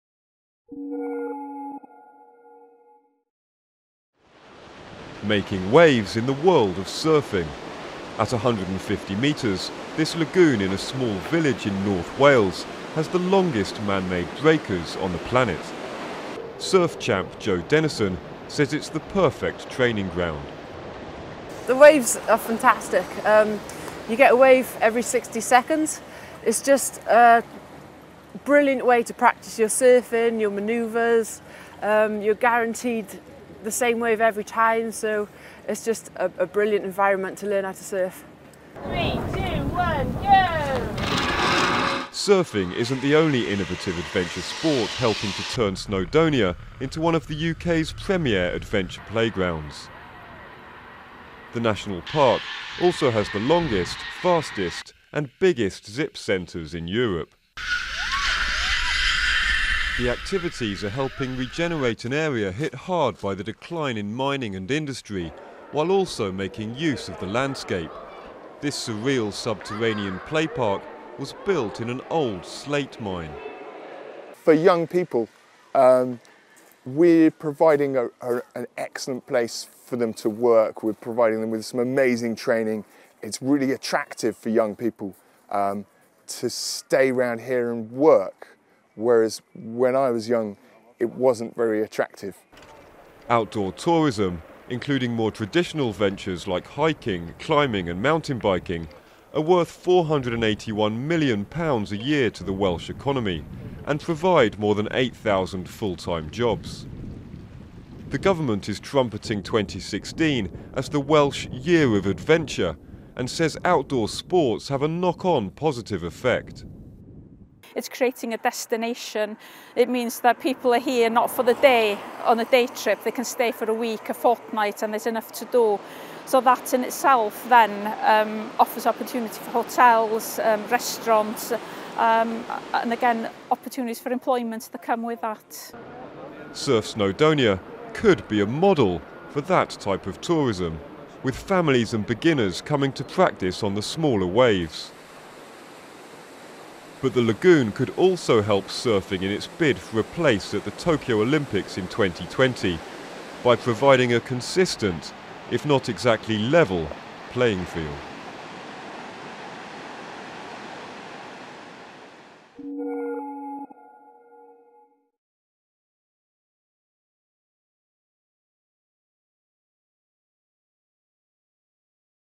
In Wales, a village catches a break with world-first surf lagoon, zip-lines and more
The opening of a new outdoor surf centre in North Wales is being hailed as a \'game changer\' for the sport, with a two-metre barelling wave crashing back and forth across an inland lagoon at the touch of a button. But it\'s also one of a number of new and unusual activities helping to revitalise the region, including Europe\'s longest and fastest zip wires and a trampoline park set in an old slate mine.